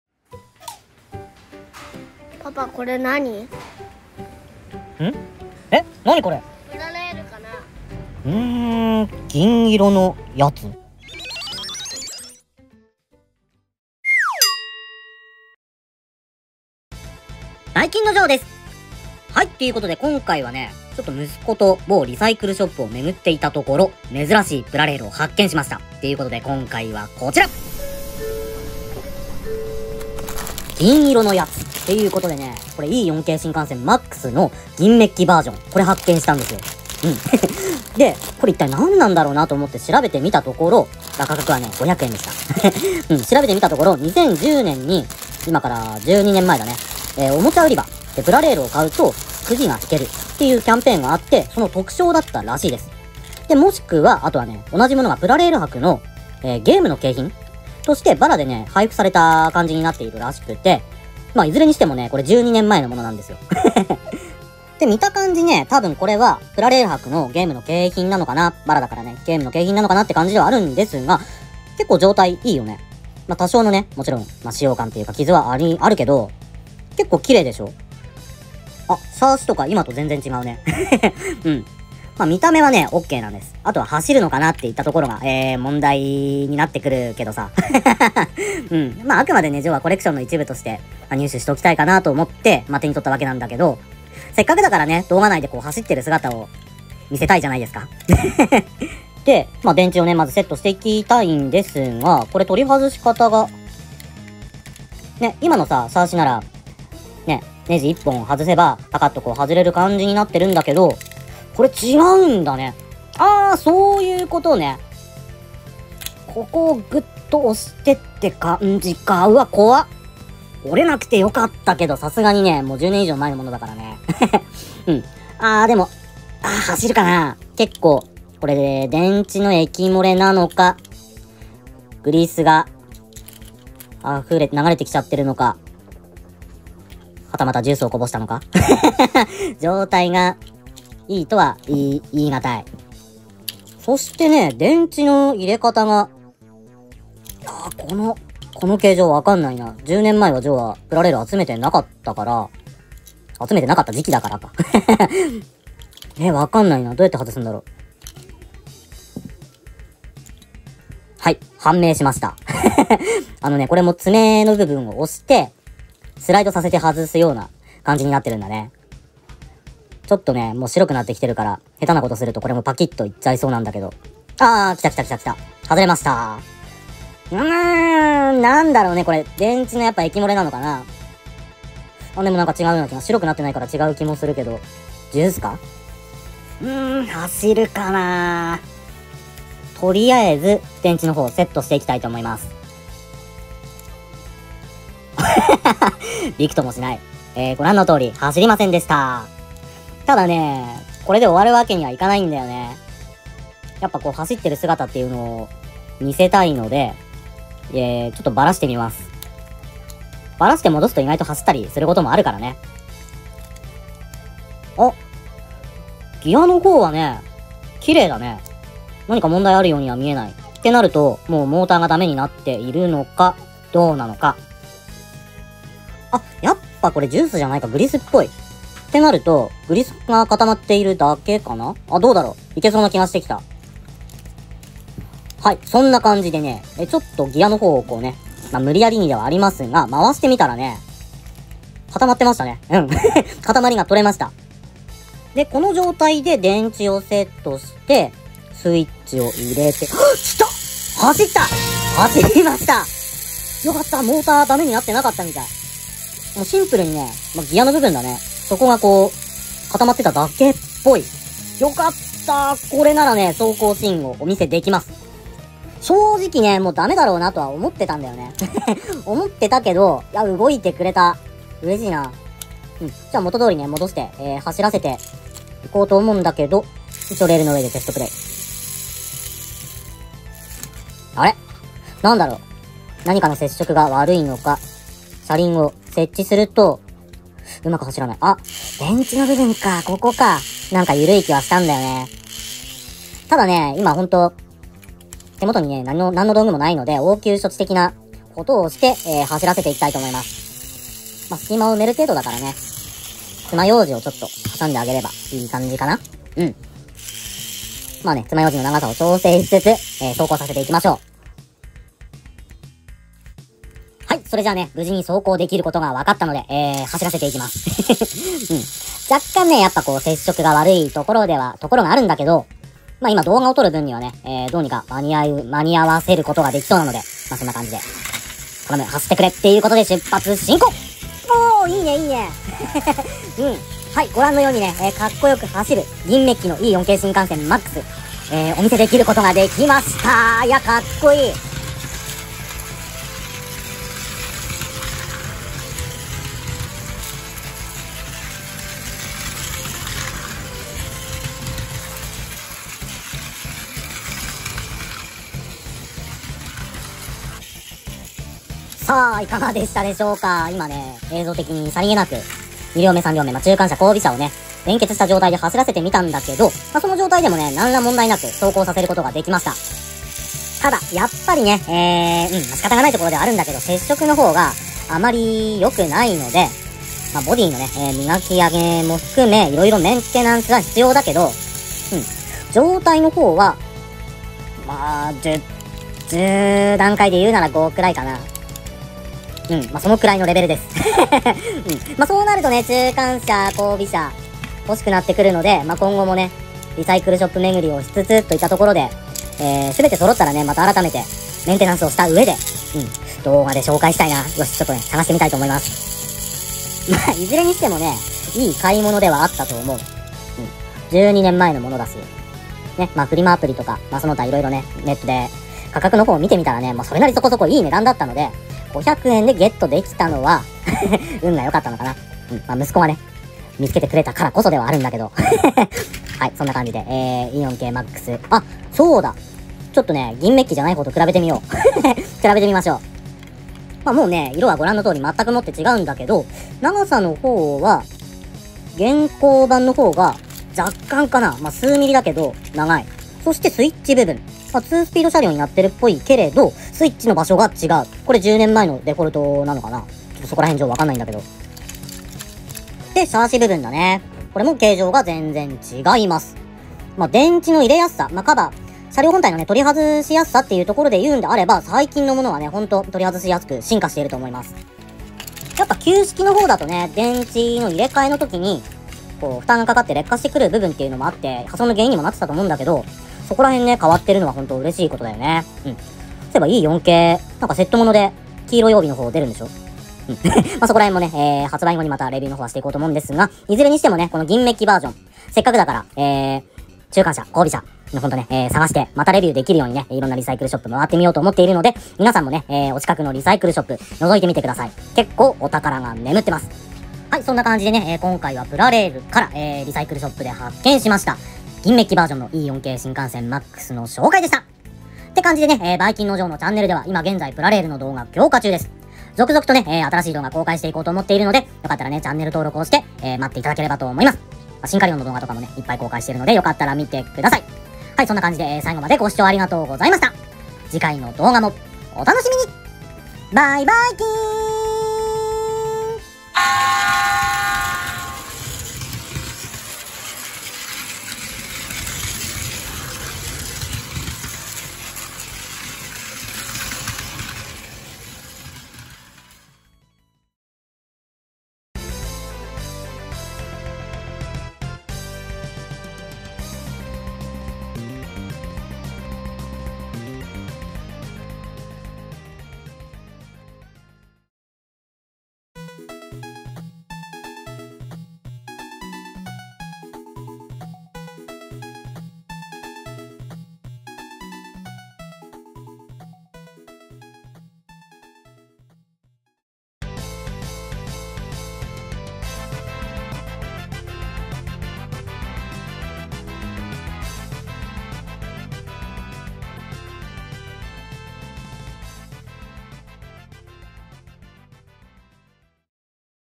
お宝発見！E4系新幹線Max銀メッキver. これ2010年のくじ引きキャンペーン特賞orプラレール博の限定品らしい Plarail Series E4 Max
プラレール、シンカリオンZを中心に玩具レビューや玩具を使用した寸劇、変形バンク再現、駅ぶら企画などいろいろやっているので良かったら他の動画もチェックしてみてくださいね！

※より鮮明に見えるよう補正している為、実際のものと見え方が異なる場合がございます。ご了承ください。

【BGM & SOUND EFFECTS】
・フリー音源サイト「魔王魂」